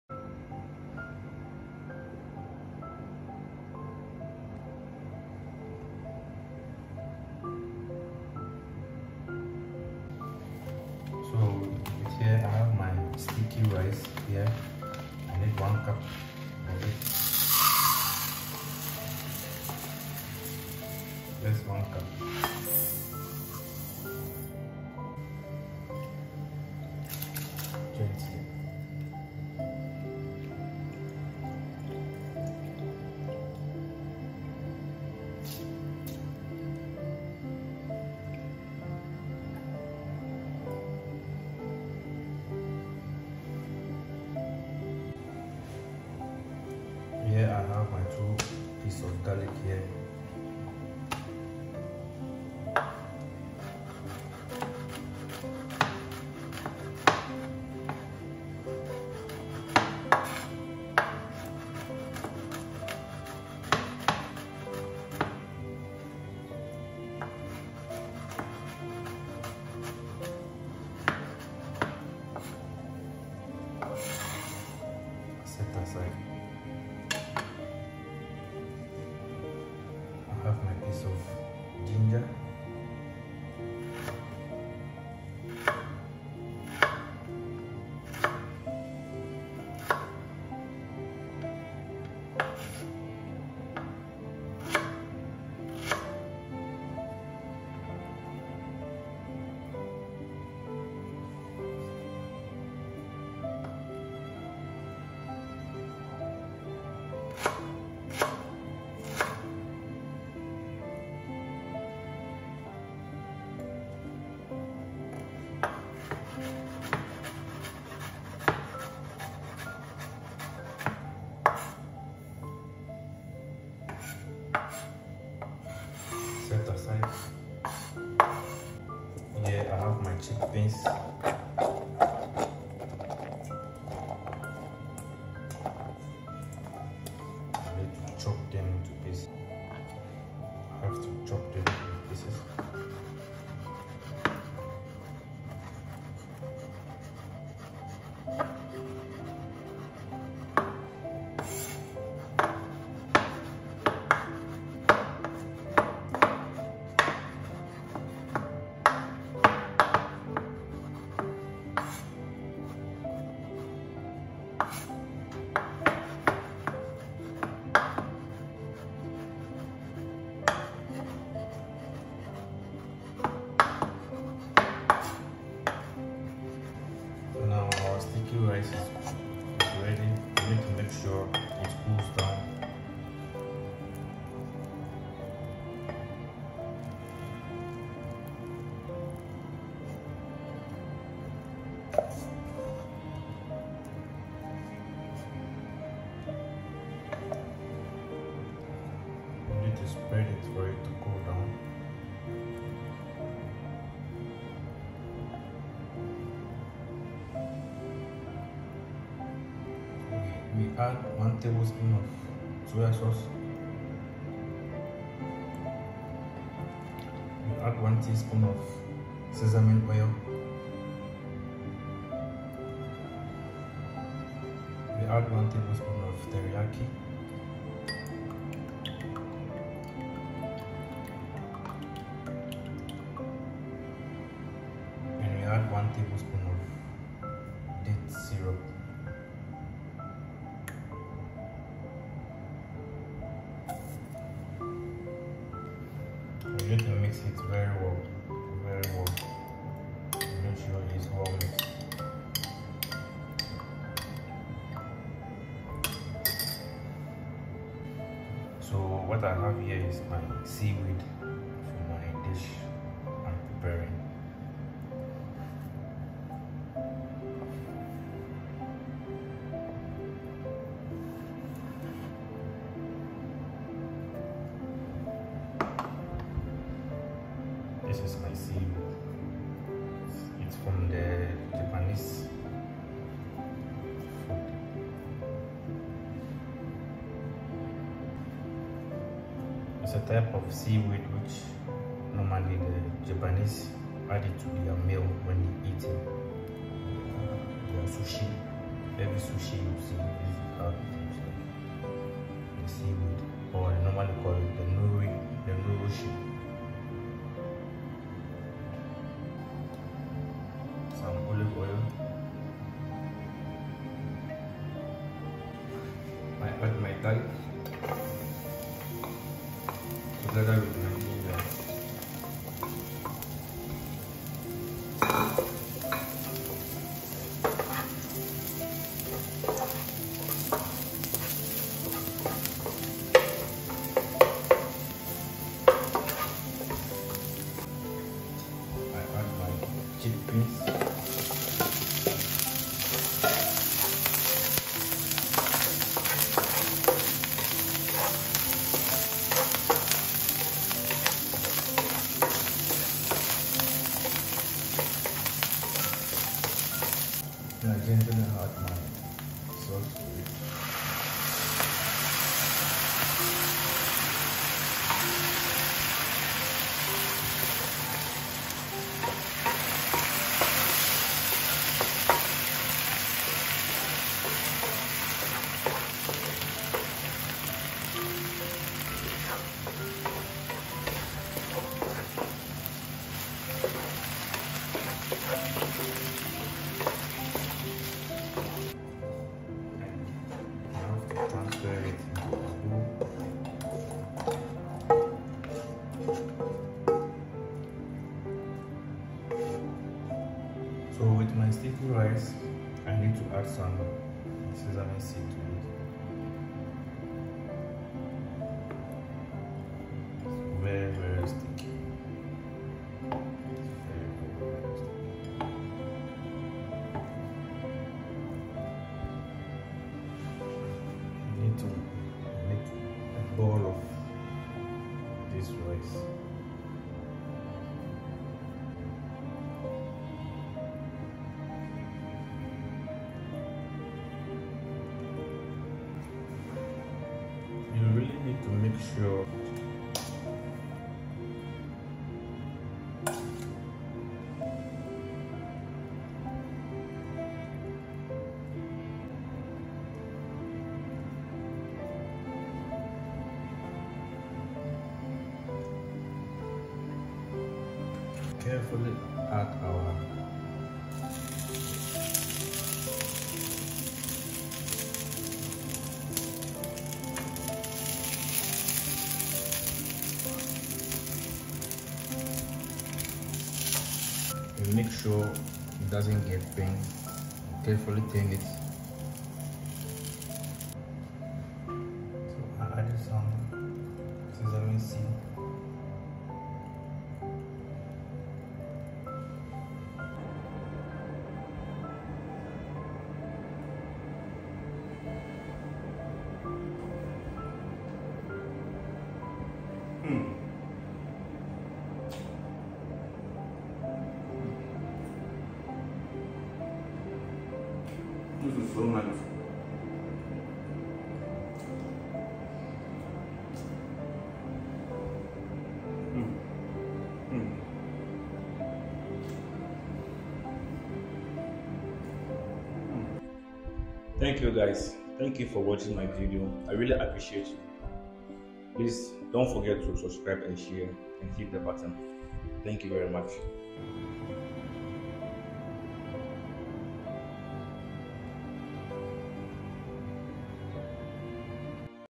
Japanese Style 🇯🇵Yaki Onigiri Rice
Learn how to prepare simple meals at home Yaki Onigiri delicious and eat with confidence.
Let’s begin
Ingredients
200g Sticky Rice
1pcs ginger
2pcs garlic

50g soy sauce
1tbsp date syrup
1tsp sesame oil
1tbsp teriyaki sauce
1tbsp rice vinegar of choice
Nori sheets/ sea weed

Facebook. Chefeat